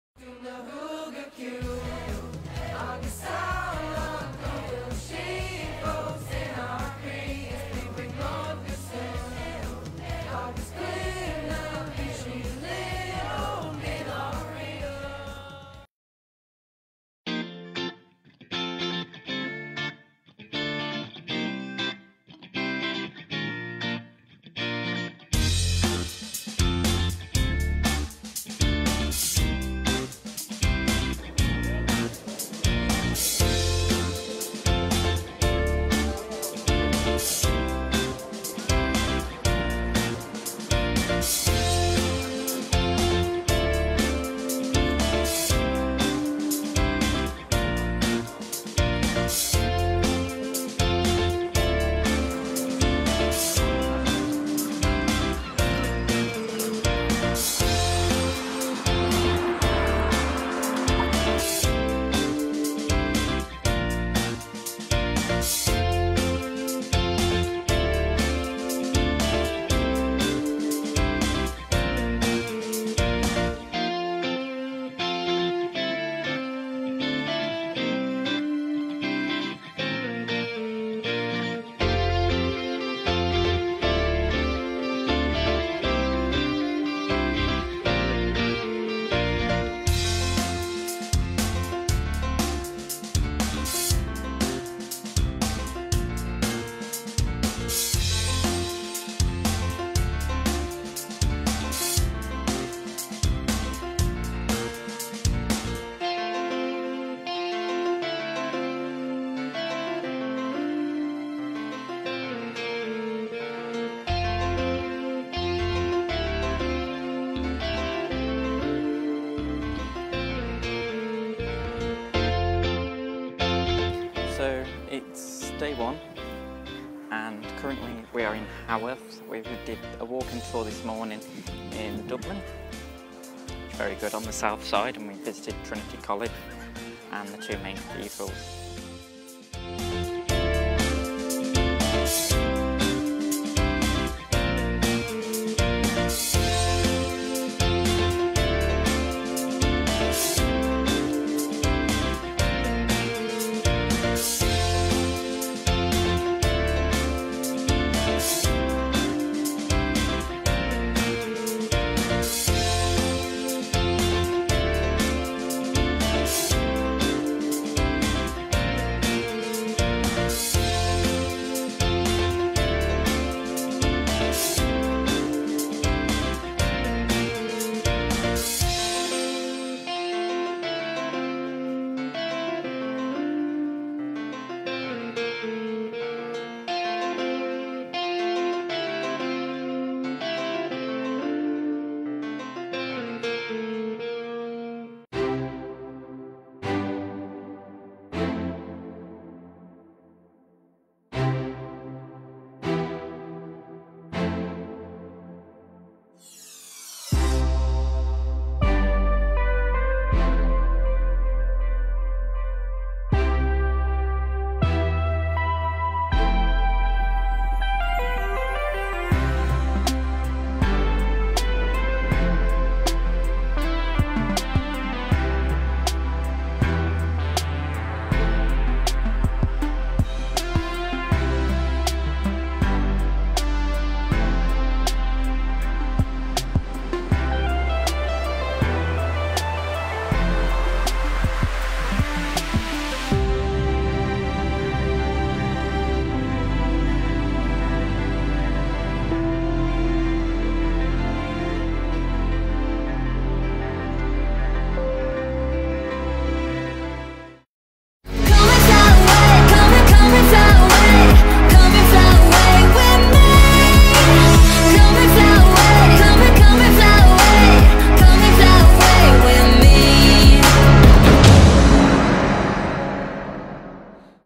Leaving Dublin Already? Dublin Montage 2017 - Day 2
Having only just arrived in the Irish Capital, I left to seek pastures new after only one day! Was it that I didn't enjoy the city after all this time? Find out in this video and the ones to follow!